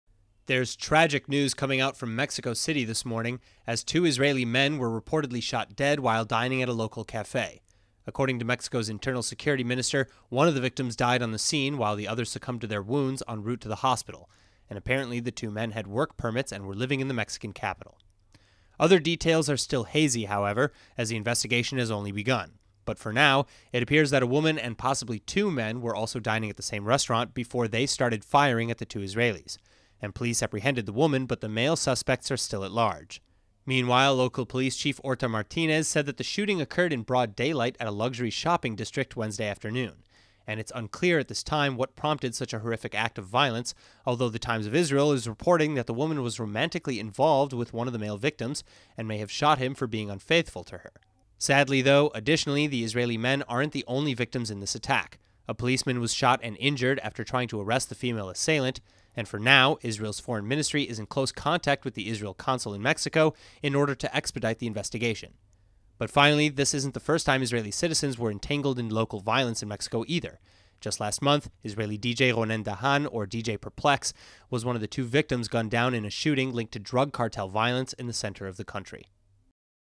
2 Israelis Shot Dead In Mexico City - Your News From Israel
Two Israeli men were shot dead while dining in Mexico City. Motive behind the broad-daylight attack not yet confirmed.